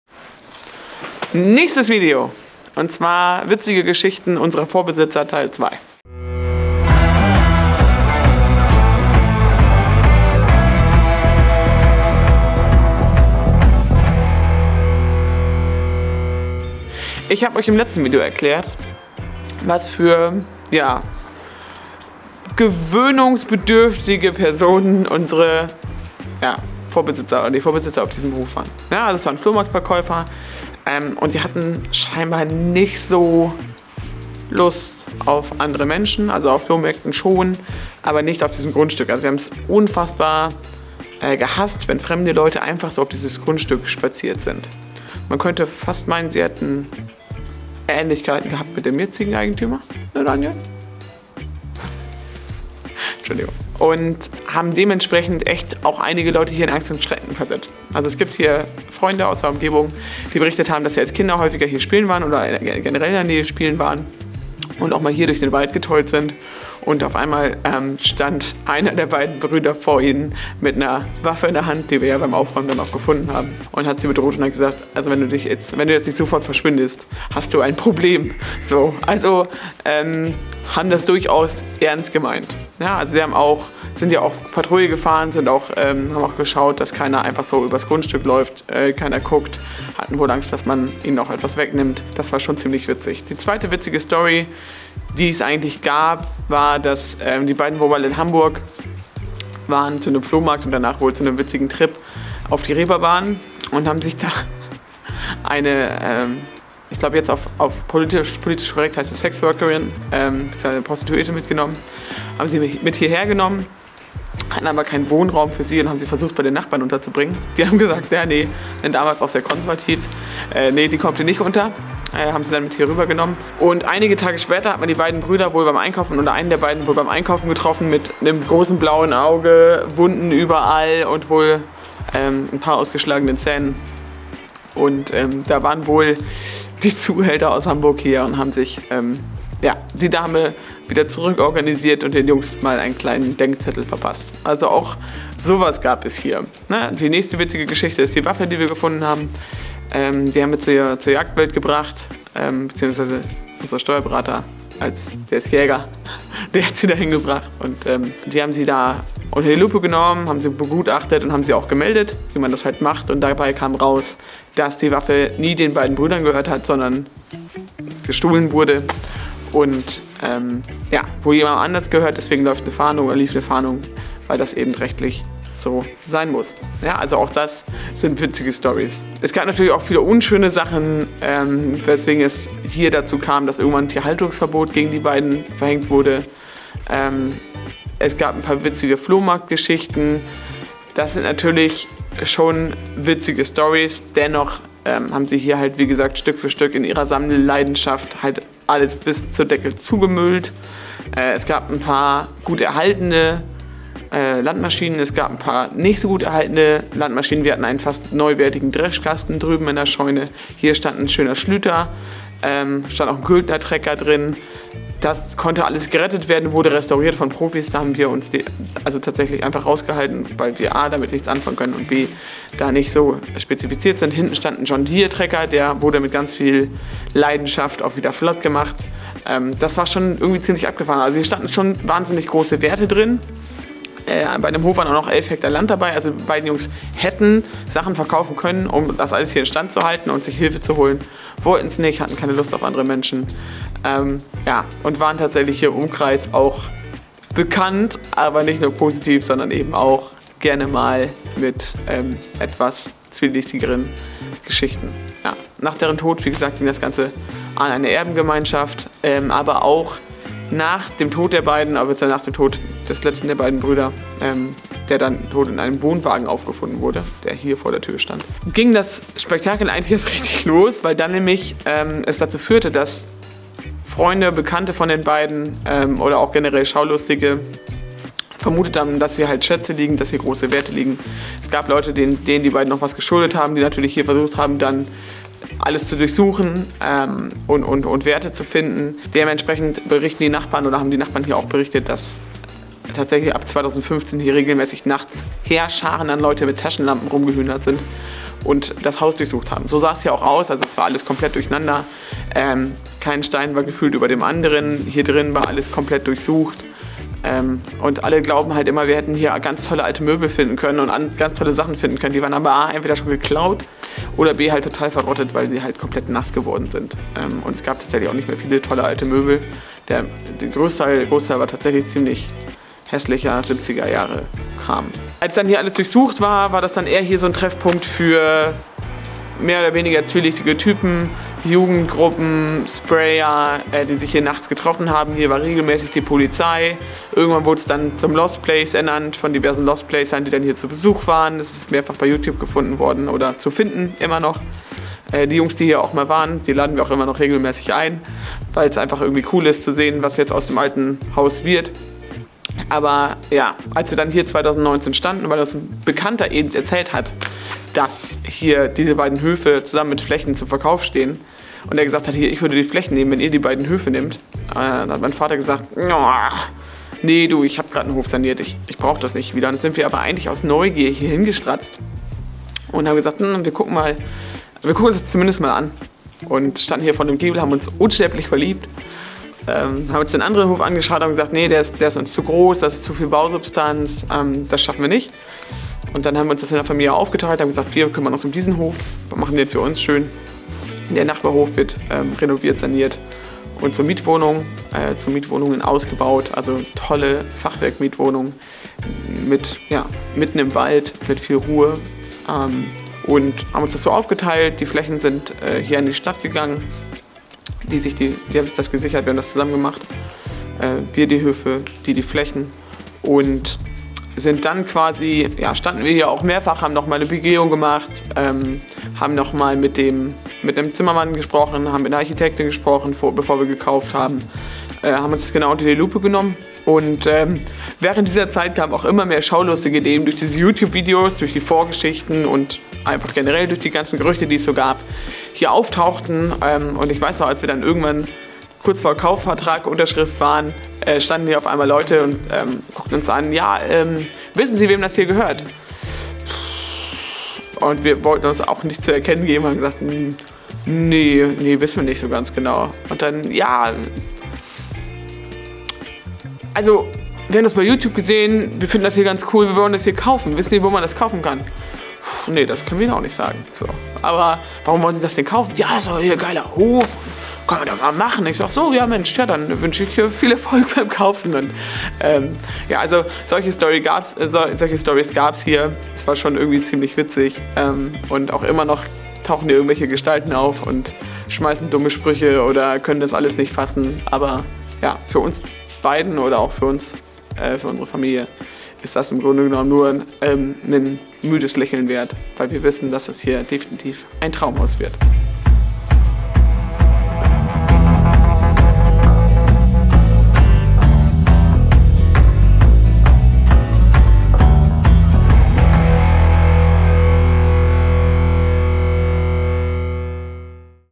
Geschichten und Anekdoten vom Hof Flatemersch
Über unseren Hof und seine Vorbesitzer gibts einige verrückte und witzige Geschichten. Vielleicht wird dir dann etwas klarer, warum der Hof so verfallen war. ;)

Du willst wissen, wie wir vorankommen?